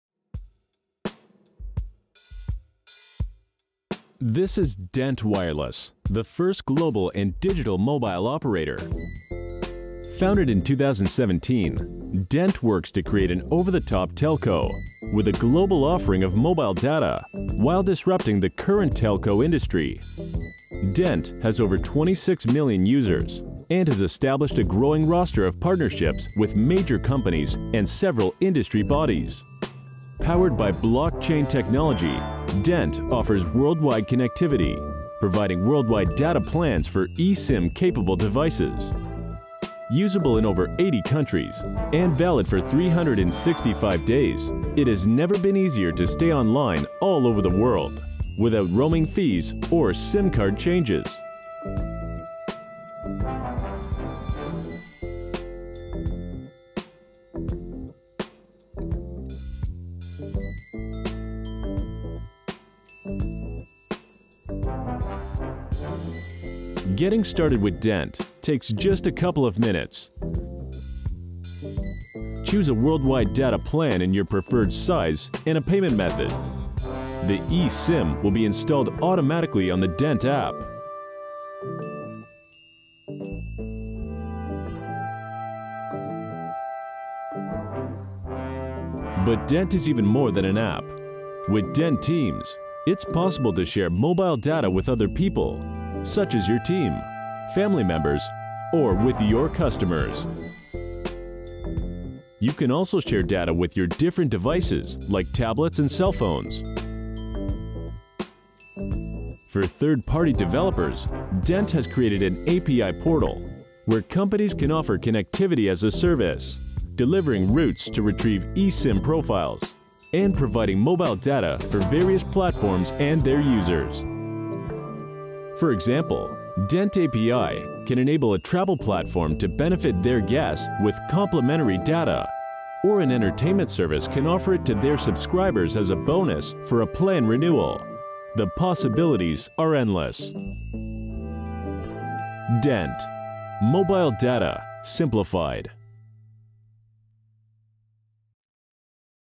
DENT Wireless – The world’s first digital and global mobile operator
Powered by Blockchain technology, DENT offers worldwide connectivity for eSIM capable devices.

🌍  More information about DENT 🌍